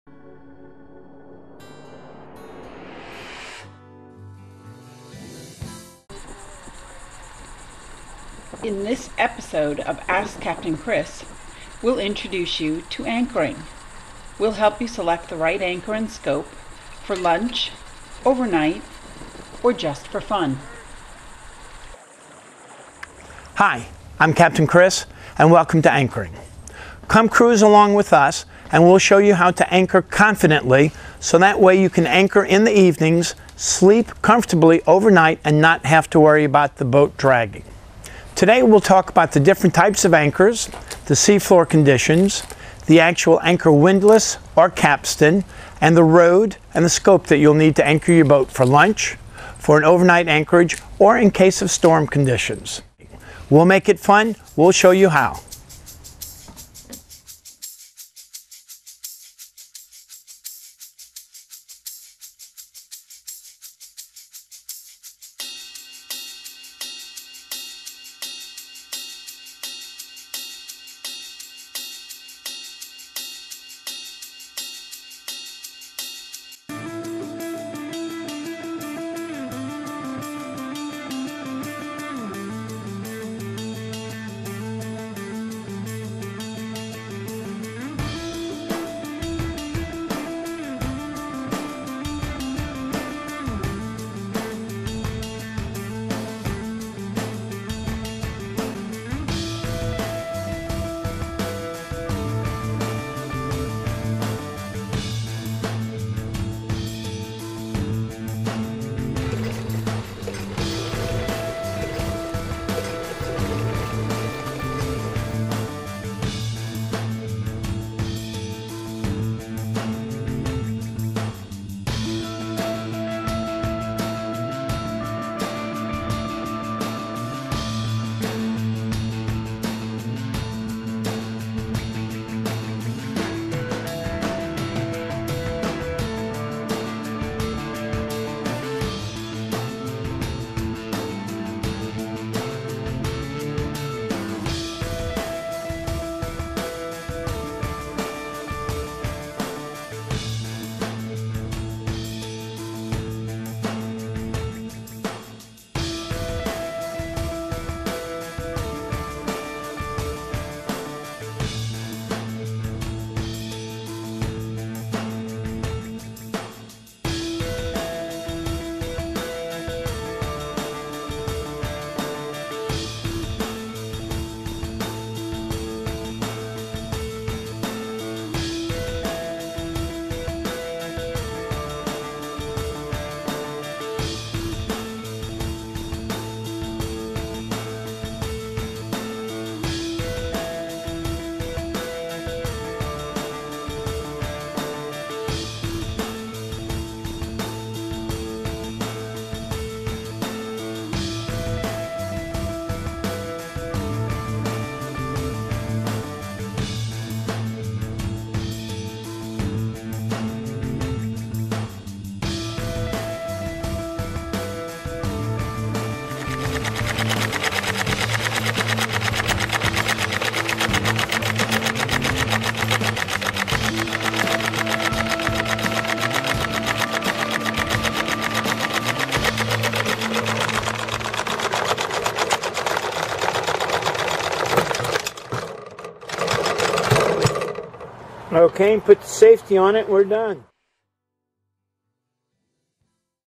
Anchoring is Easy with Captain Chris
Captain Chris will put your mind at ease the next time you set your hook. Our latest video in the Ask Captain Chris training series - ANCHORING - is now available at our ships store.

If you have ever dragged your anchor or listened to anchor horror stories from your boating buddies then this video will help you anchor like a pro. Learn about different anchor designs and which anchors are right for the sea floor conditions where you plan to be. We'll make it easy to determine your scope and length of rope or chain and how to do it all without yelling!

Ask Captain Chris about ANCHORING...he will show you how and you won’t have to clean mud off the deck.